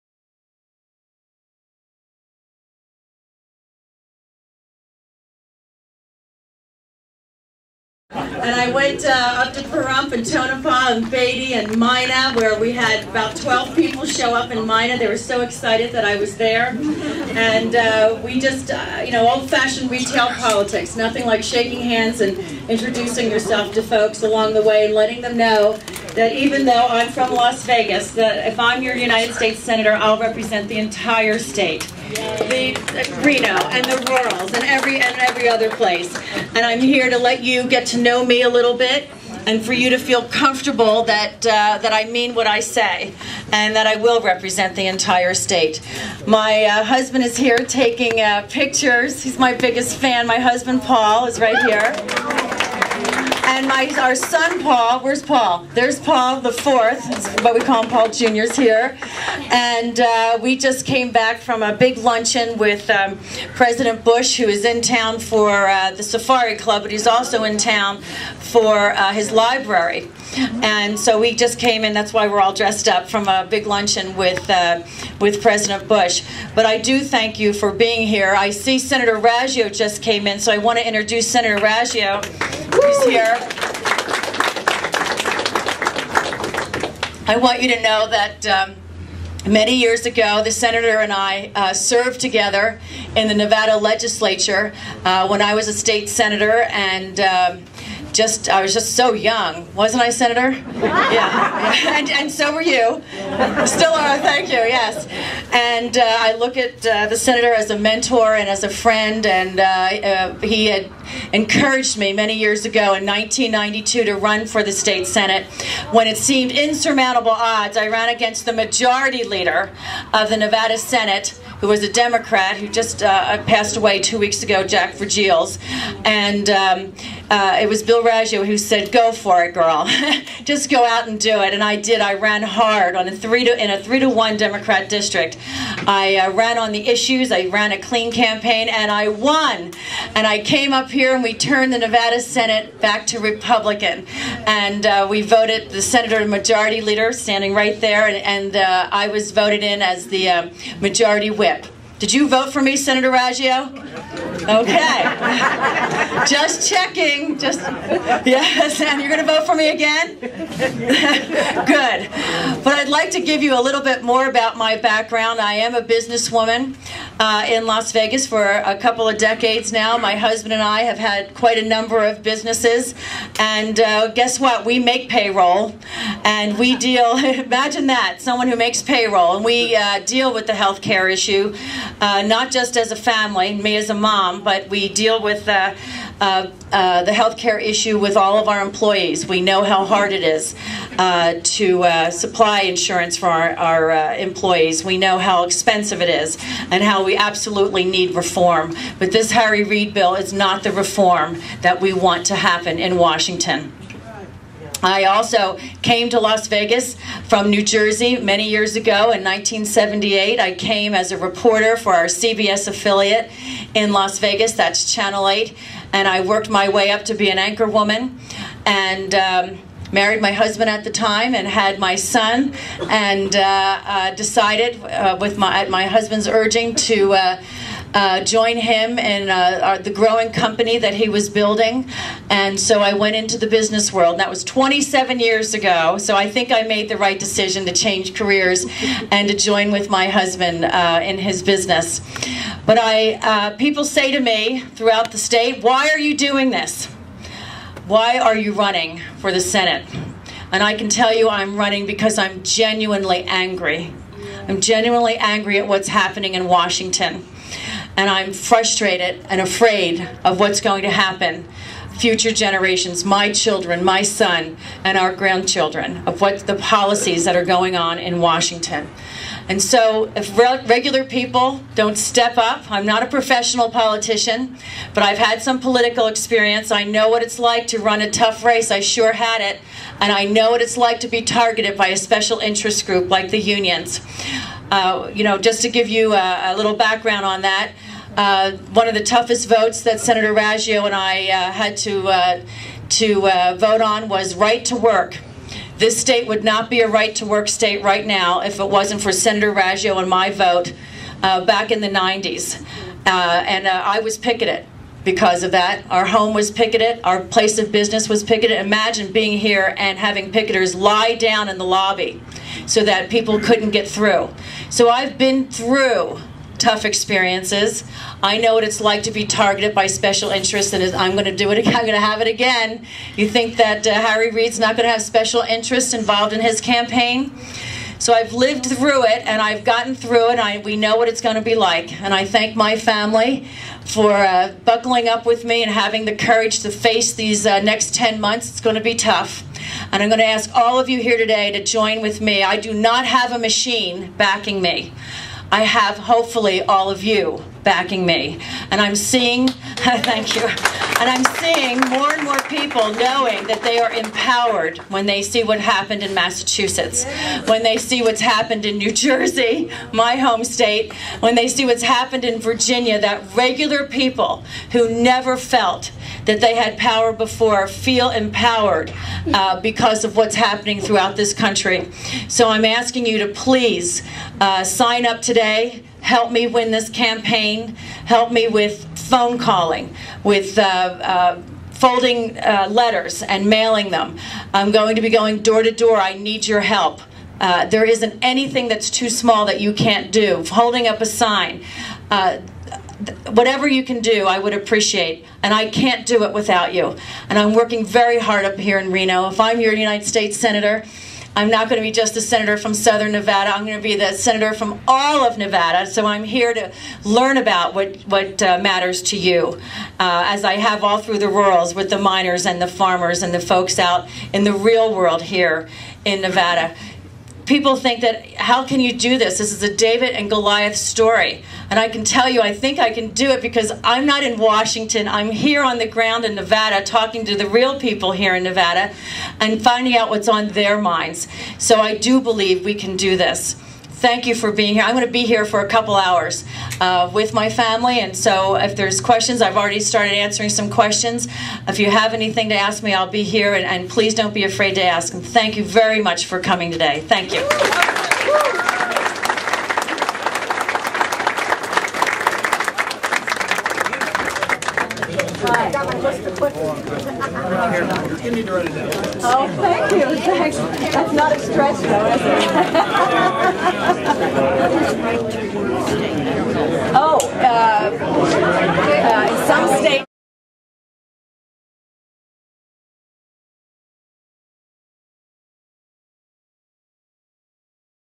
US Senate Candidate Sue Lowden Opens Reno Campaign office and Kicks off Her Campaign
US Senate Candidate
Sue Lowden's
Reno, NV Campaign Office Opening/Official Campaign Kickoff on 1.23.10
Filmed by: The VoiceBox Radio Show